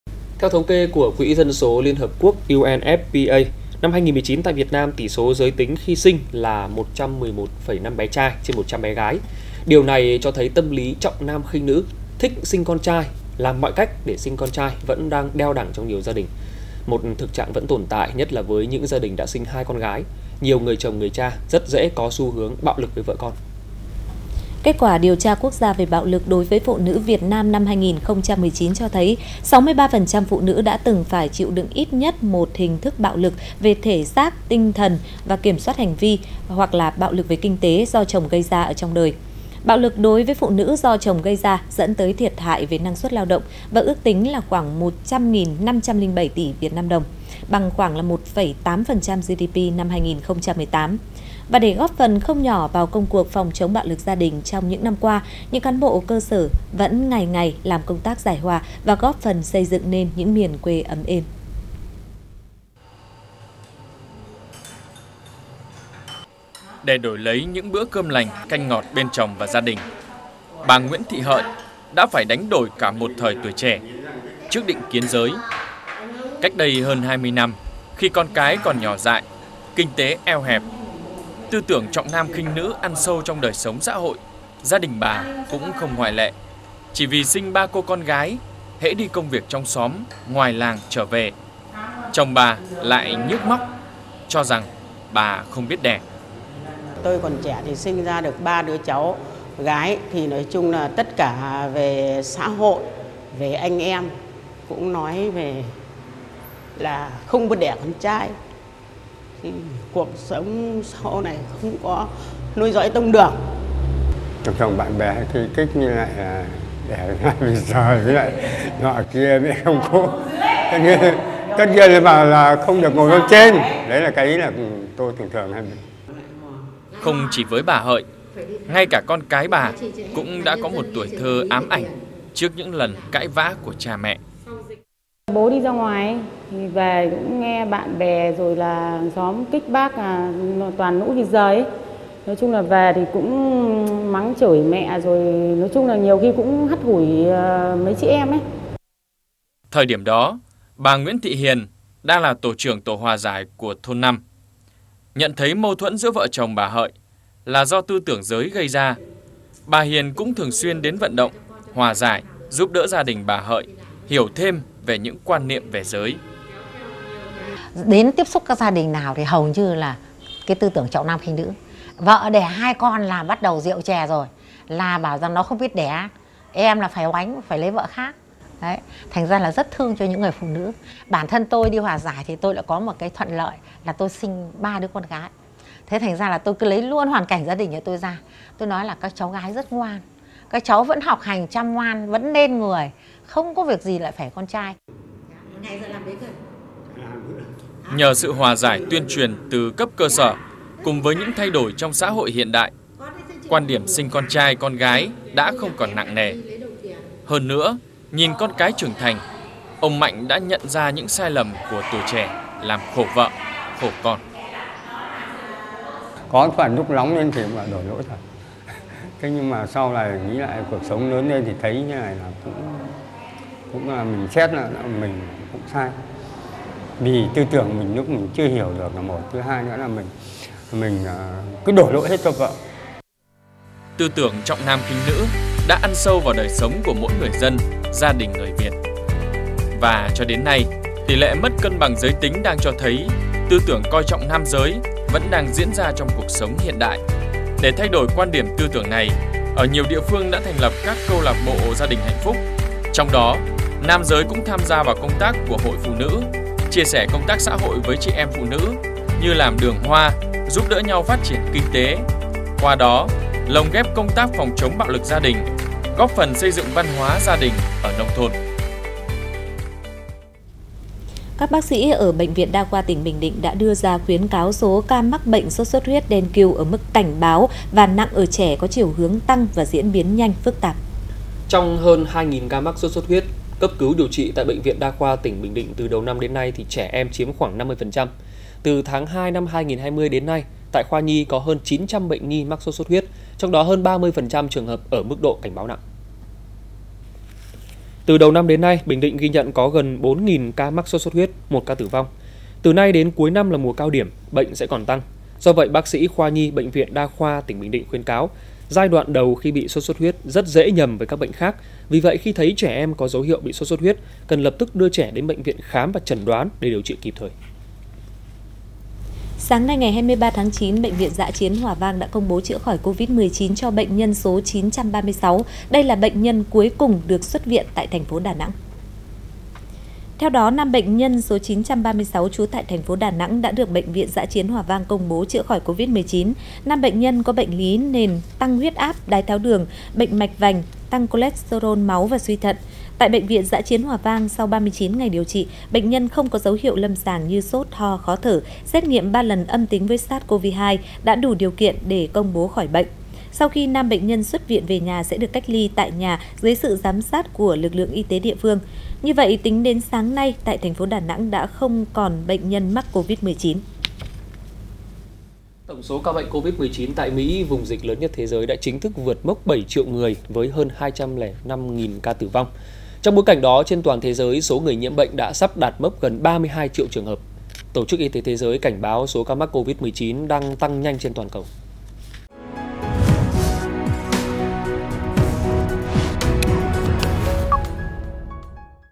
"Khát" con trai: Gốc rễ của bạo lực gia đình | VTC16
VTC16 | Dù đã là thế kỷ 21 nhưng tâm lý trọng nam khinh nữ, thích sinh con trai vẫn đang đeo đẳng trong nhiều gia đình. Việc sinh toàn con gái, khiến nhiều người chồng, người cha rất dễ có xu hướng bạo lực với vợ con.